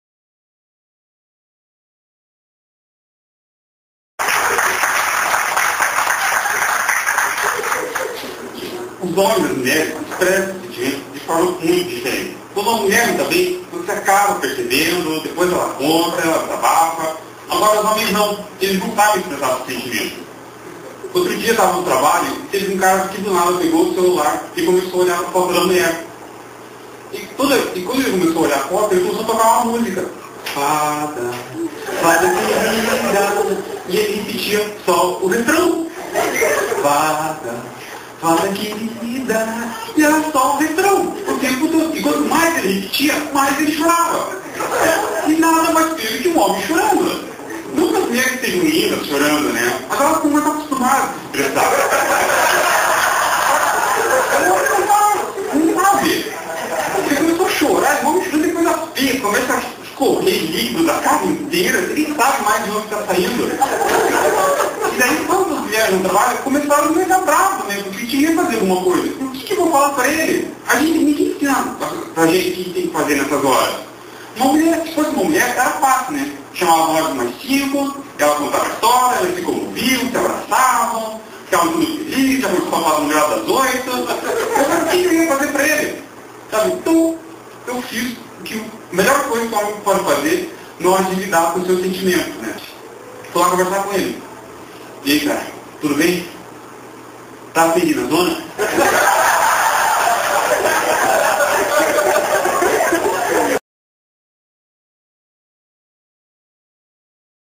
Stand Up Comedy com ISMAEL CHRISTINO
"Os homens e as mulheres expressam seus sentimentos de forma diferente."

Apresentações no Era Só O Que Faltava todos os sábados às 21h (Curitiba/PR).